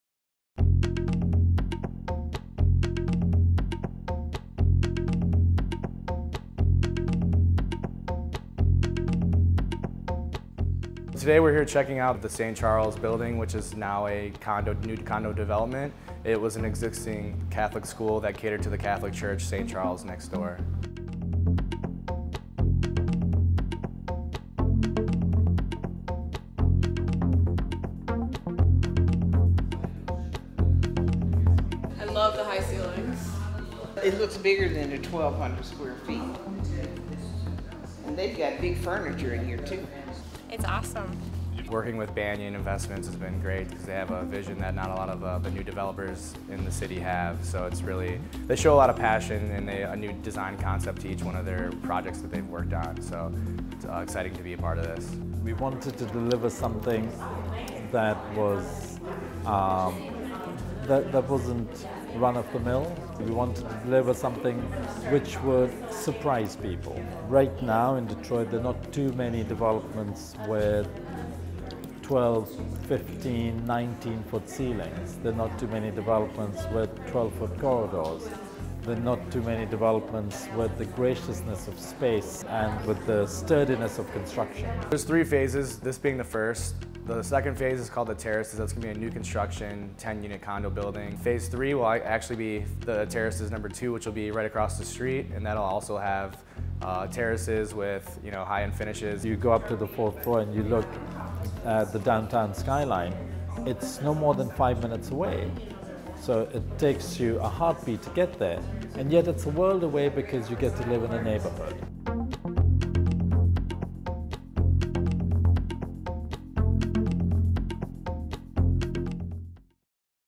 Saint Charles Condos in Islandview: Model Unit Reveal & Aerial Tour
Join us for an exclusive walkthrough of the brand new model unit at Saint Charles Condominiums in Islandview! Discover the stunning upscale finishes, featuring 14-foot ceilings and exceptional craftsmanship. Take a sneak peek at the penthouse level from above, offering breathtaking views and a glimpse into the luxury living that awaits. Whether you're exploring the spacious interior or admiring the modern design, this video will give you a first look at what makes Saint Charles Condos truly unique. Don’t miss out on this exciting reveal!